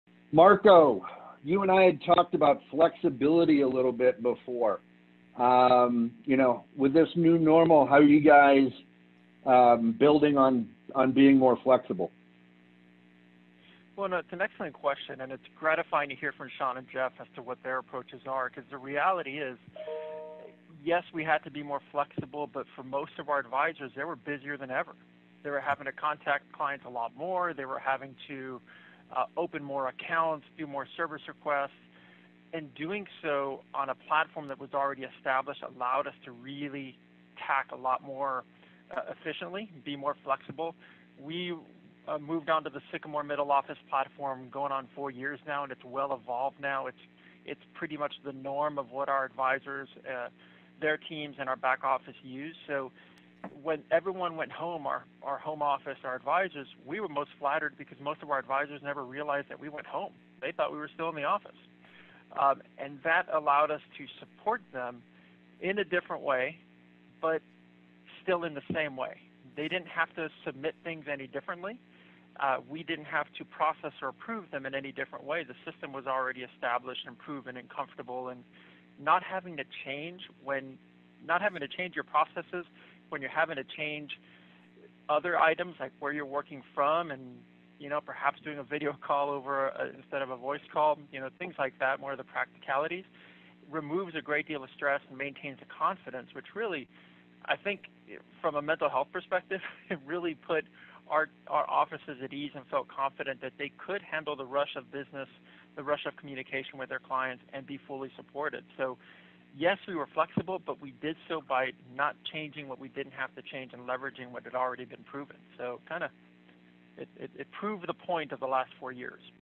Marco Galvan of Prospera Financial Services: Remote Work Flexibility
In this interview series on “What We Learned by Going Remote,” Chip Kispert of Beacon Strategies interviews Marco Galvan, Director of IT and Platform at Prospera Financial Services, who explains how the need for flexibility and their use of the Sycamore platform – already established across advisors, their teams and the back-office – allowed the firm to tackle more work, more efficiently in a remote work environment.